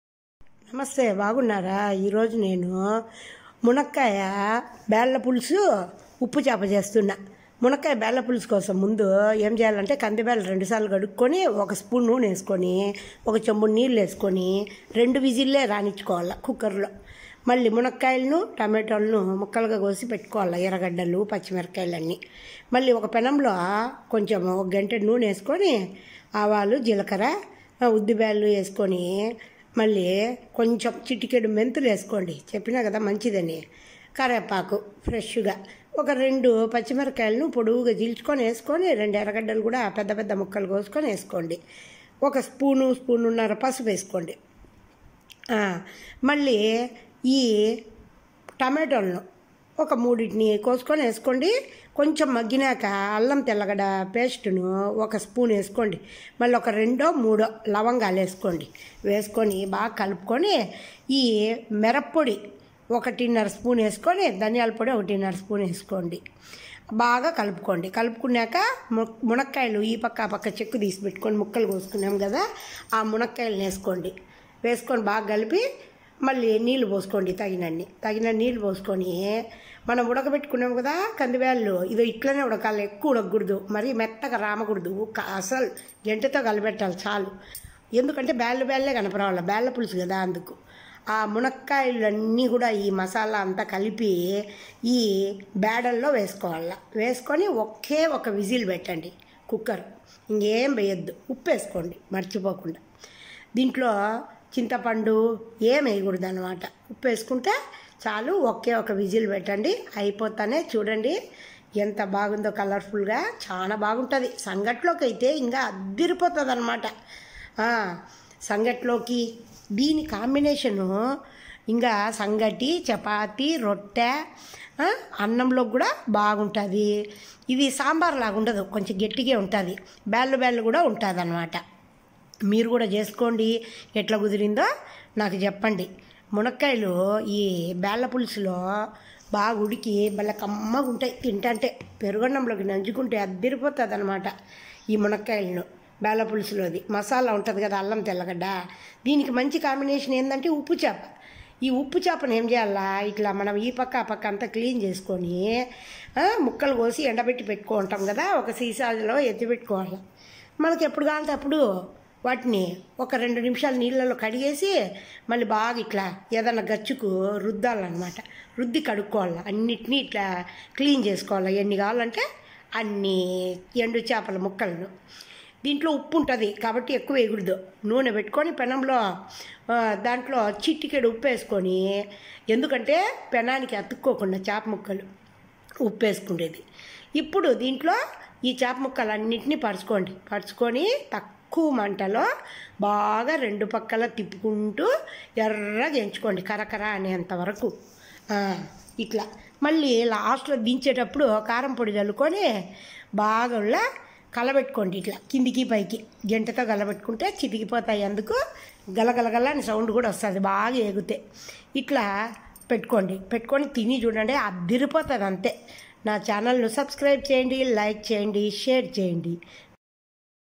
Uppu chepa, munakkaya bedala pulusu | ఉప్పుచేప, మునక్కాయ బేడల పులుసు.
ఈ మునక్కాయ పులుసు లో కి ఉప్పుచేప చాలా బాగుంటుంది.  మంచి కాంబినేషన్ ఇది
తప్పకుండ ట్రై చేసి ఎలా ఉందొ కామెంట్ చెయ్యండి